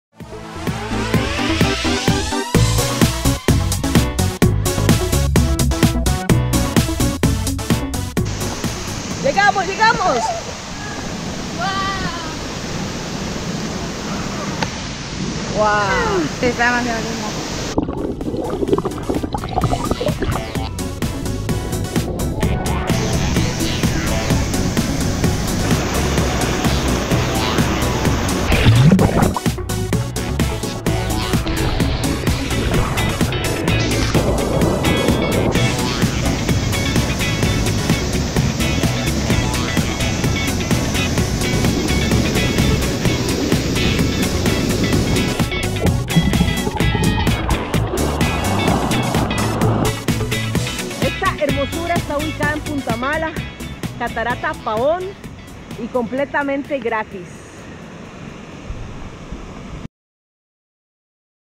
Catarata Pavón.
Esta Catarata llamada Pavón está ubicada en Punta Mala de Uvita.
Muy fácil de llegar y Completamente GRATIS.
Después de disfrutarla puedes pasar al Restaurante de Tilapias que está en la salida de esa bella Catarata y almorzar delicioso.